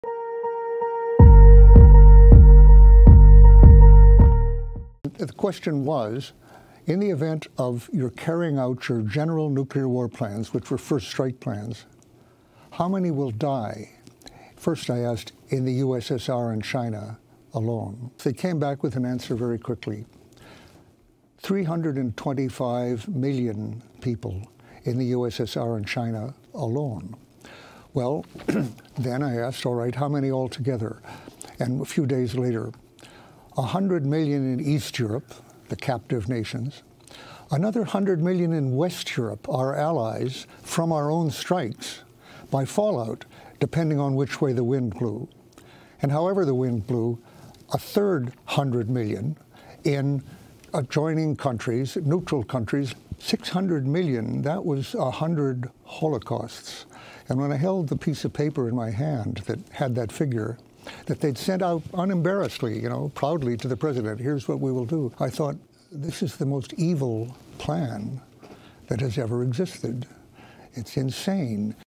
Ellsberg: Un ataque nuclear contra la URSS en 1961 habría causado el equivalente a cien holocaustos
Daniel Ellsberg se hizo famoso por filtrar los documentos conocidos como Papeles del Pentágono, que contribuyó a destituir al presidente Nixon y acelerar el fin de la guerra en Vietnam, pero también dedicó su vida al activismo antinuclear, luego de haber trabajado en 1961 para el Gobierno estadounidense en la planificación de guerras nucleares.  Según Ellsberg, los cálculos del Gobierno de EE.UU. mostraban que la cifra de muertos que provocaría un ataque nuclear a la Unión Soviética era seiscientos millones de personas, el equivalente a “cien holocaustos […] una locura”, dijo Ellsberg en una entrevista con Democracy Now! en 2017.